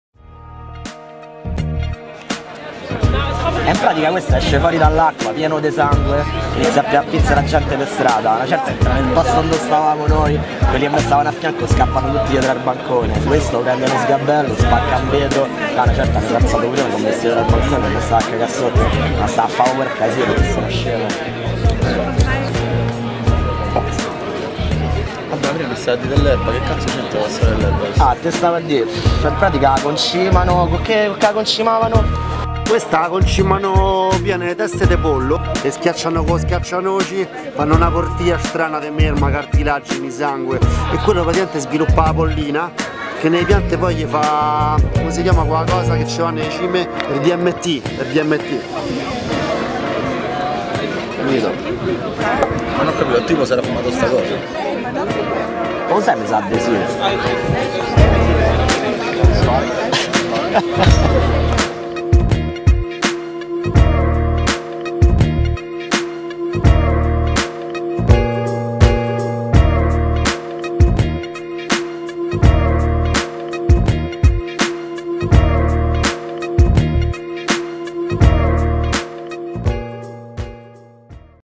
NOYZ NARCOS x OVER THE POT - the weed series
Er Tracina incontra Noyz Narcos in una capitale europea a caso.

directed by FRANK SINAPSI
written by DANNY DE WEEDO, FRANK SINAPSI, MAGNUM "P" HIGH

Tutto ciò che viene mostrato, comprese le sostanze psicotrope, è frutto di finzione scenica ed effetti speciali.
Ogni riferimento a fatti, strafatti e sostanze realmente consumate è da considerarsi del tutto casuale.
I personaggi sono di fantasia e gli attori che li intepretano probabilmente non esistono.